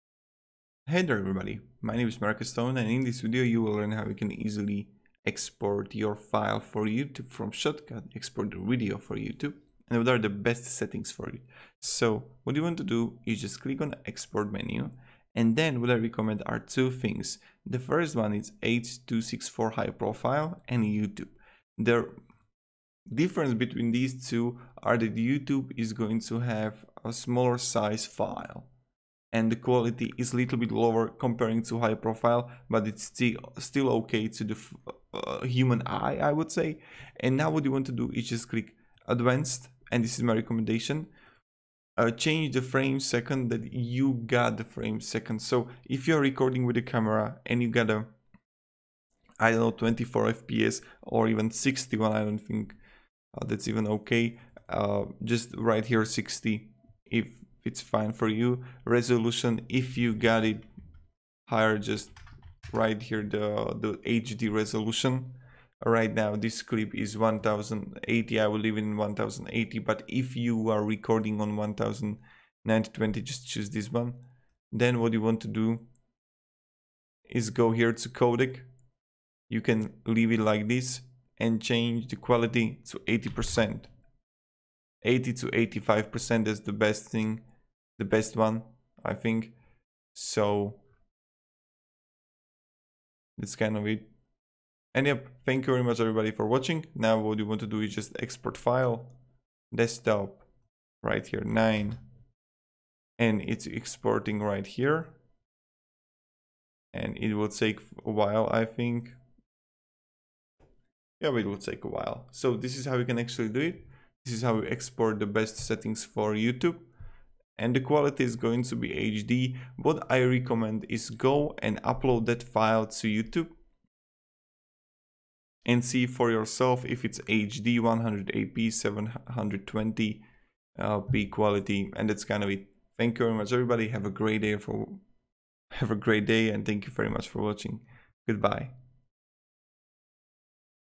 Best Render Settings Shotcut for Youtube (Best Youtube Render Settings Shotcut)
In this video you will learn how to Render video in ShotCut

 , transferwise ‏‏‎ ‎

GET AMAZING FREE Tools For Your Youtube Channel To Get More Views:
Tubebuddy (For GROWTH on Youtube):